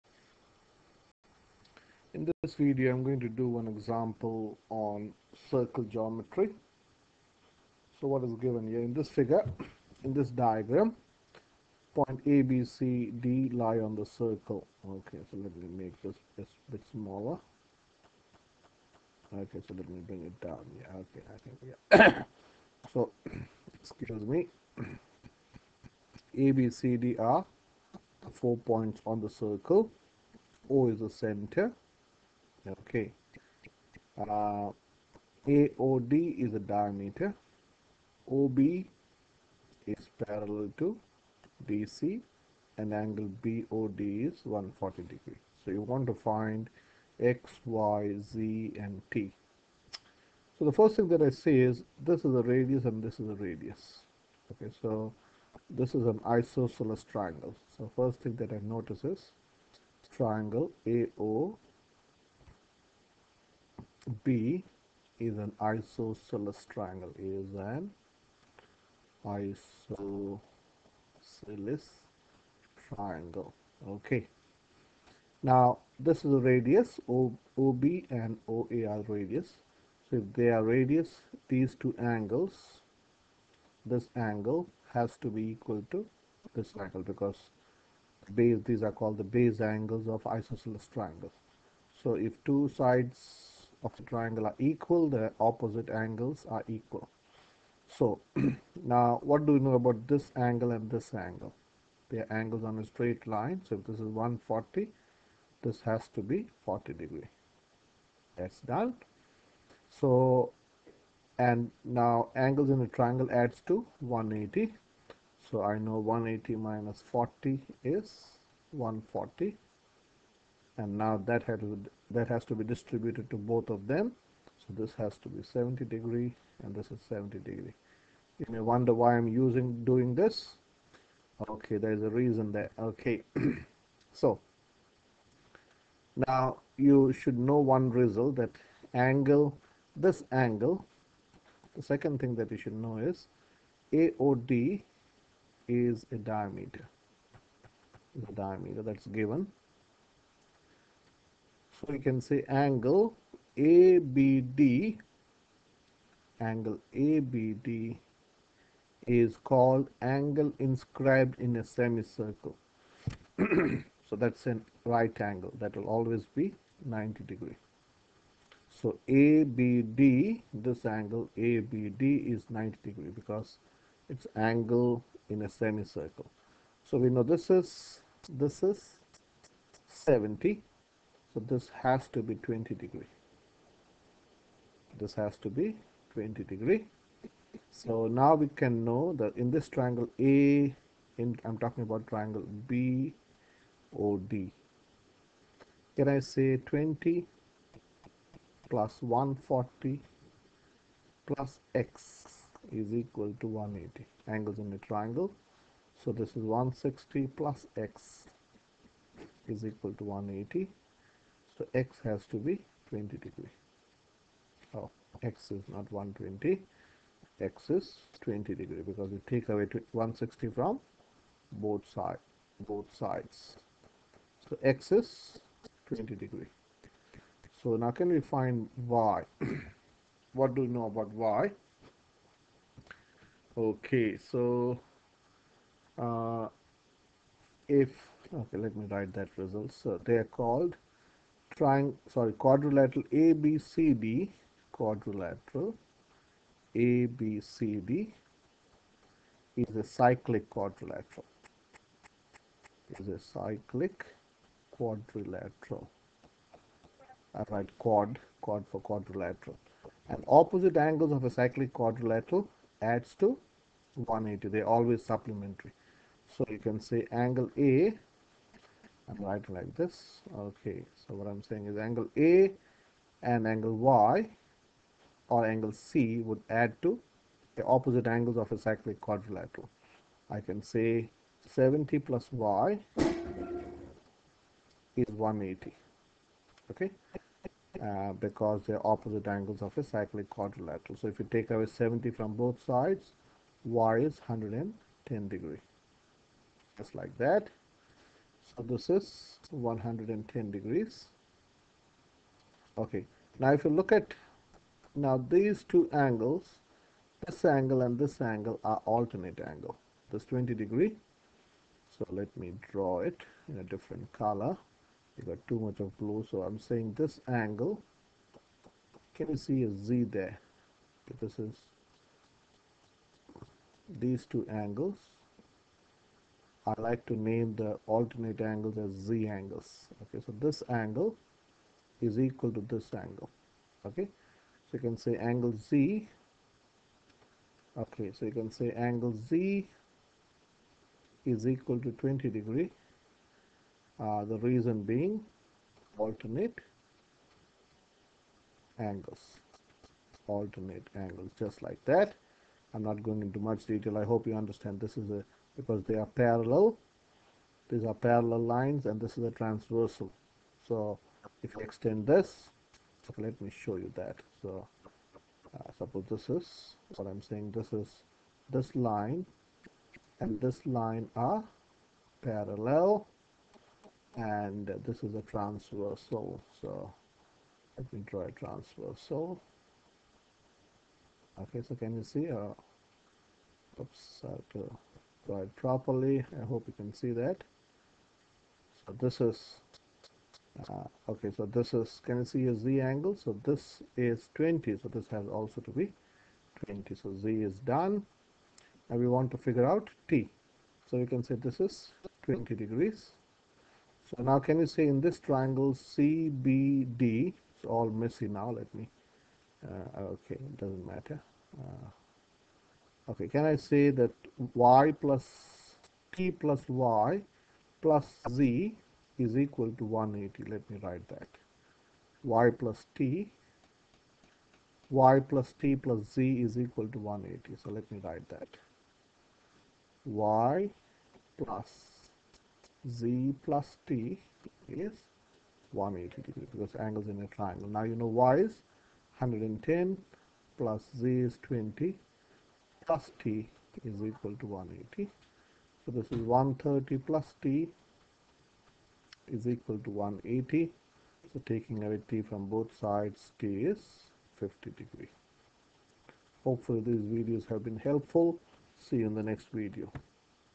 NCEA Level 1 Geometric reasoning
In this video I have shown how to use the different rules of Circle Geometry to find the measure of angles formed in a circle.